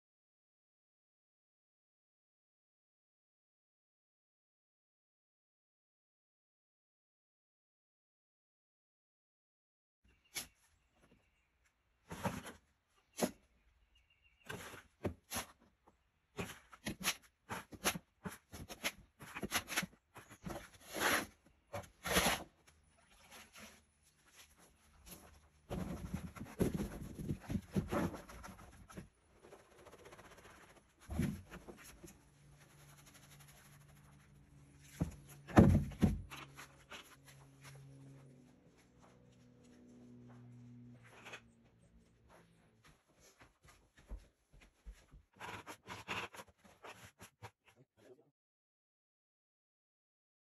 Mixing Quikrete Bags without making a mess | time-lapse
Mixing sand/topping mix in a bucket for a mudfloat inside a shower floor